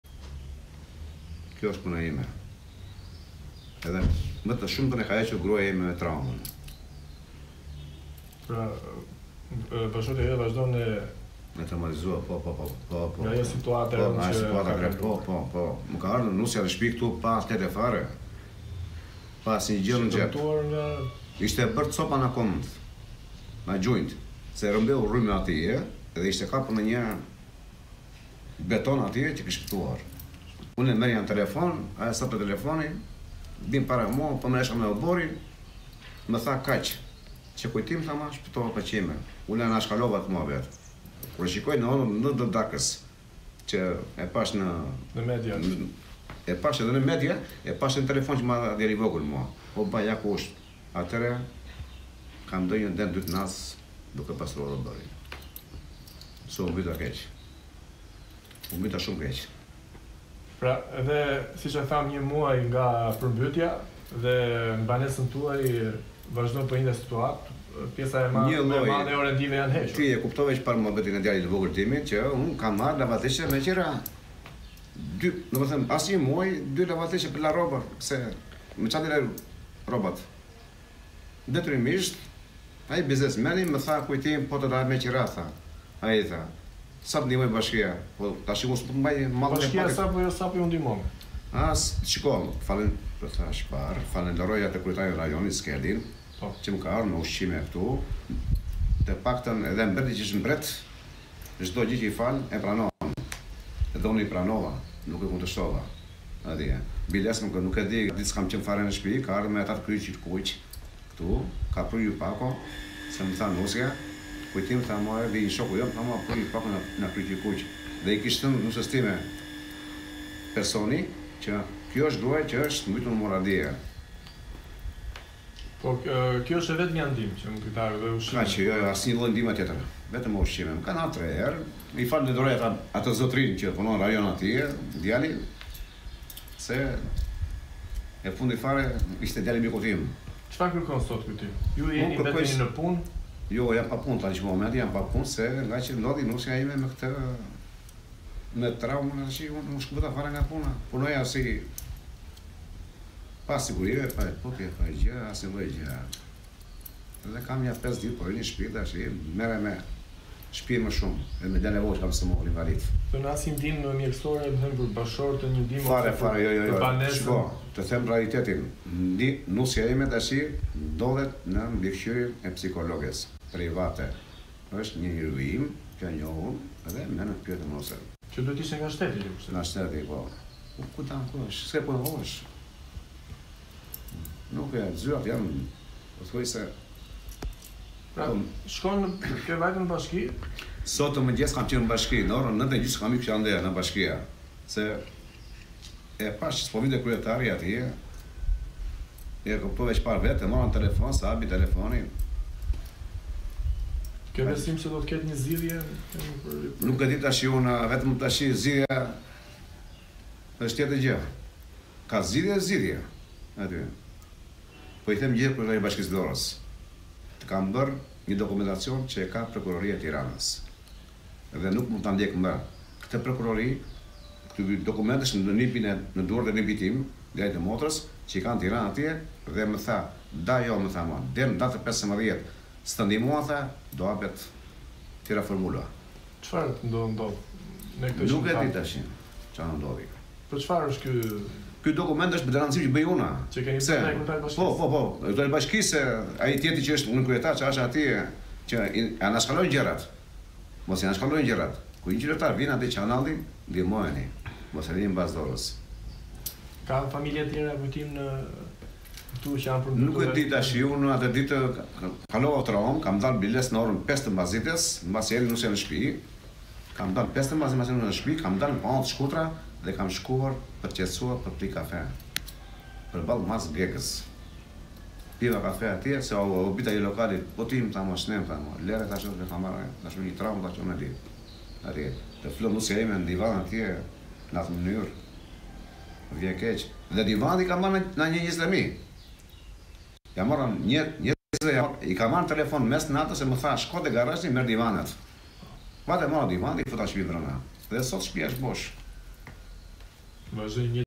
'E kam shtëpinë bosh', banori: Nuk kam asnjë orendi në shtëpi, dua dëmshpërblim
Bashkia Vlorë braktis banorët e përmbytur